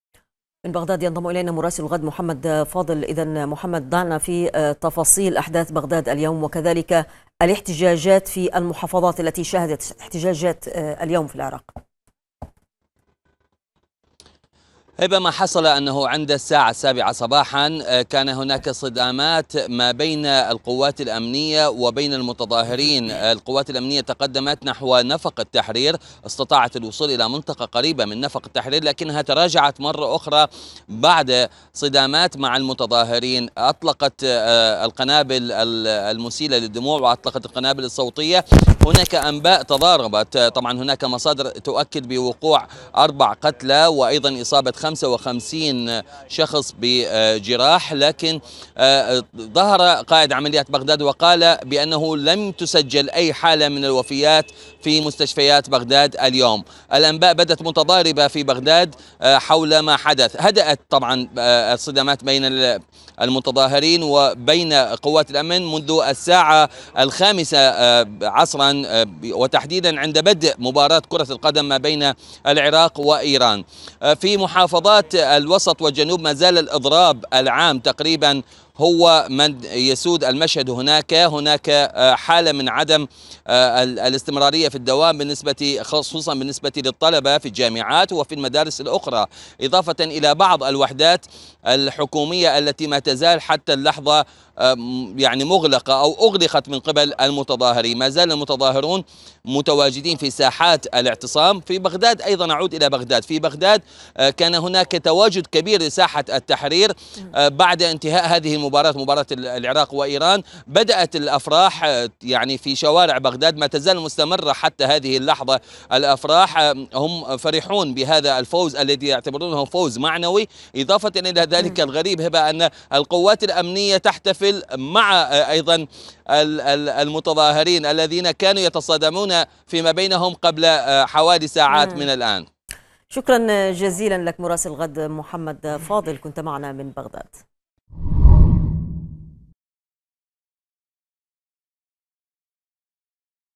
احتفالات في بغداد بعد فوز المنتخب العراقي على إيران .. مراسلنا يرصد الأجواء
قالت الشرطة_العراقية ومسعفون إن قوات الأمن قتلت أربعة محتجين وأصابت العشرات في بغداد اليوم أثناء محاولاتها دفع المحتجين للرجوع إلى مخيمهم الرئيسي في ساحة التحرير وسط بغداد. كما شهدت عدة محافظات أخرى لاسيما البصرة والديوانية والمثنى احتجاجات لمئات الطلبة وسط انتشار قوات الأمن العراقية... المزيد من التفاصيل يتحدث من بغداد محمد فاضل، مع هبة الغمراوي، عبر الفقرة الإخبارية.